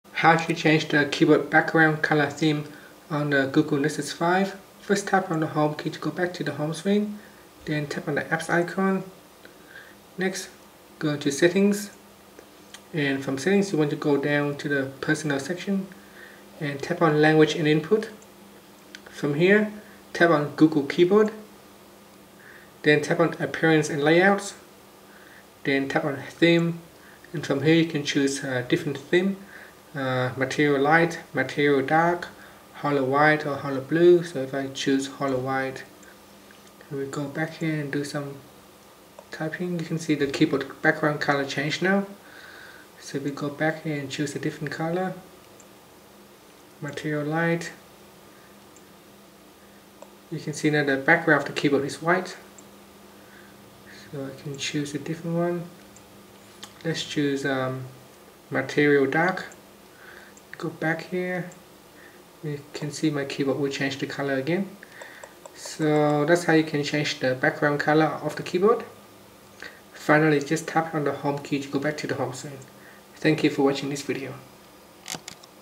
Google Nexus 5: How to Change Keyboard Background Color Theme
Learn how you can change the keyboard background color theme on Google Nexus 5.